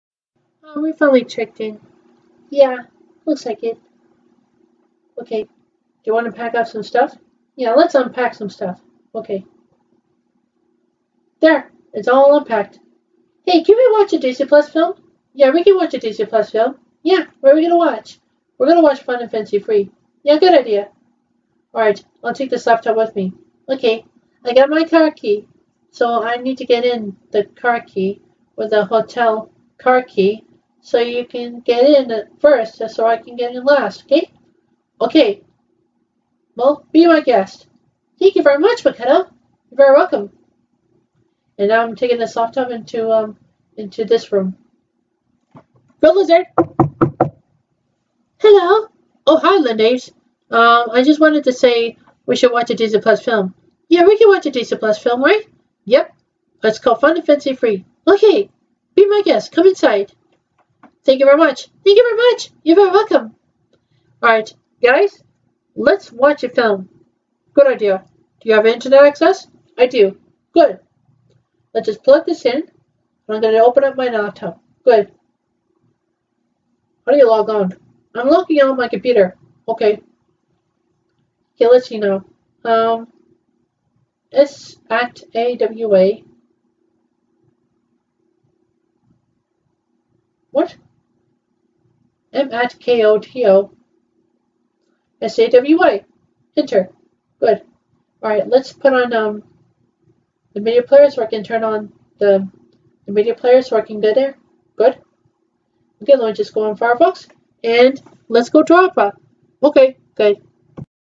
Holiday Inn Express Hotel San Francisco California USA 2021
It Was Nice Inside And We Can Stay For 3 Nights.